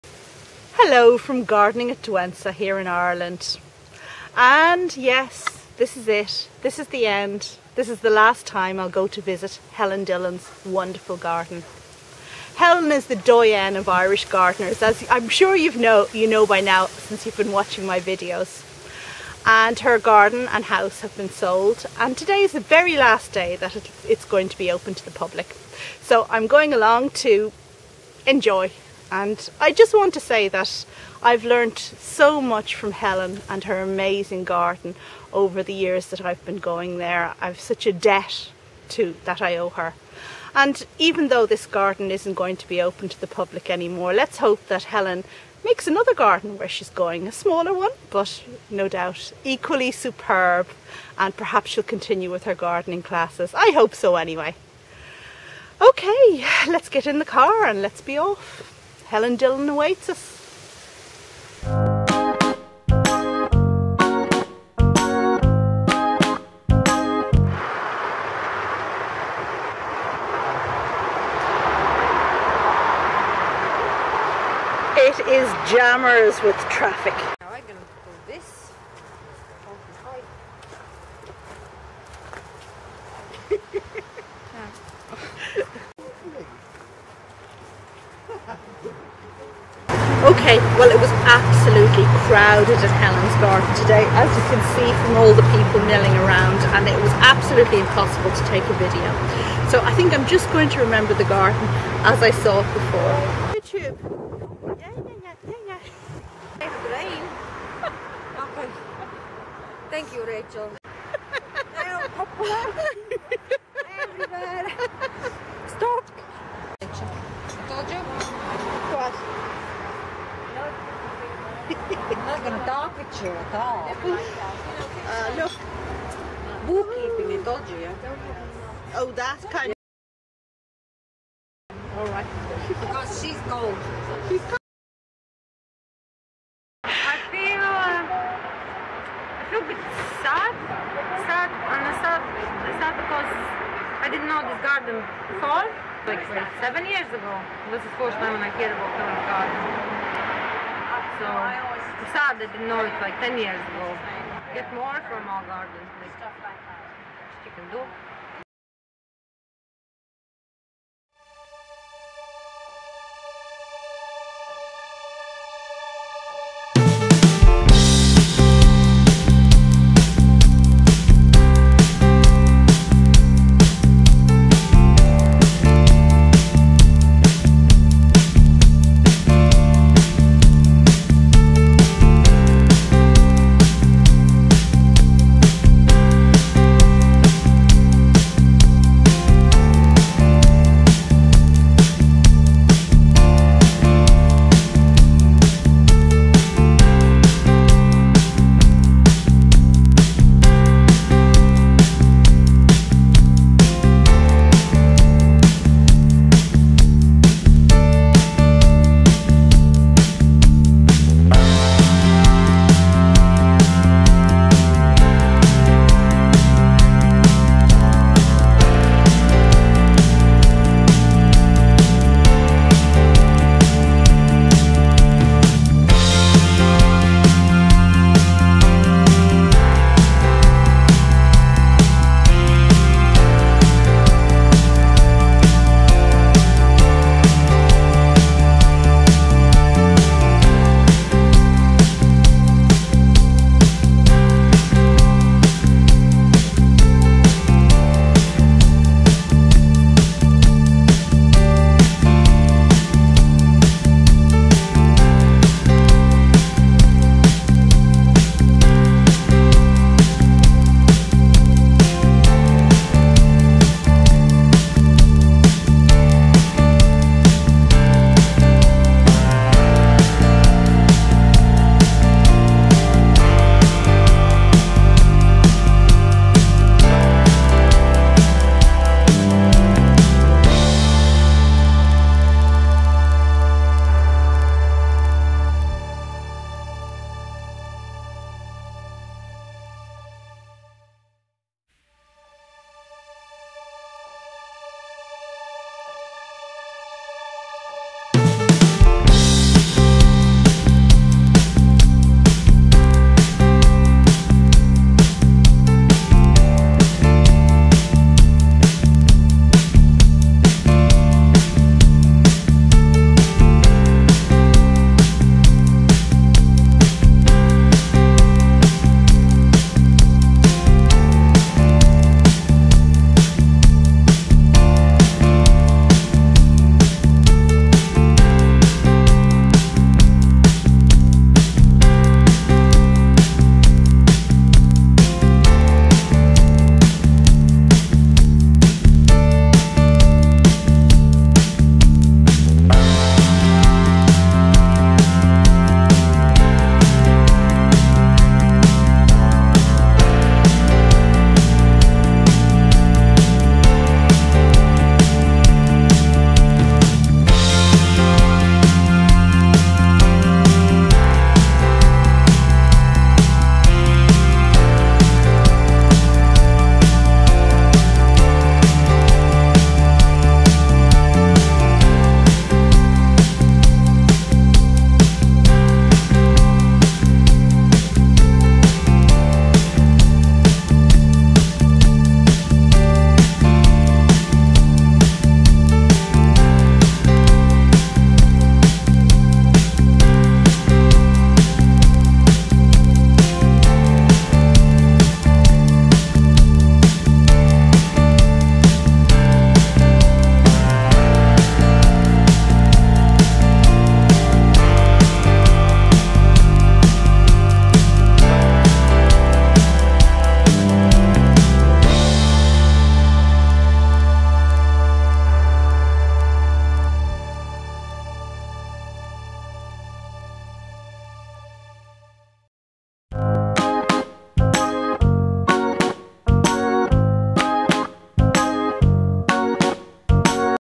Goodbye Dillon Garden
If you liked this video, you'll love my book, Journal of an Irish Garden, with forward by Helen Dillon.

It is the end of an era. The Dillon Garden has closed. This video was taken on 25 September 2016, the last day that the garden was open to the public.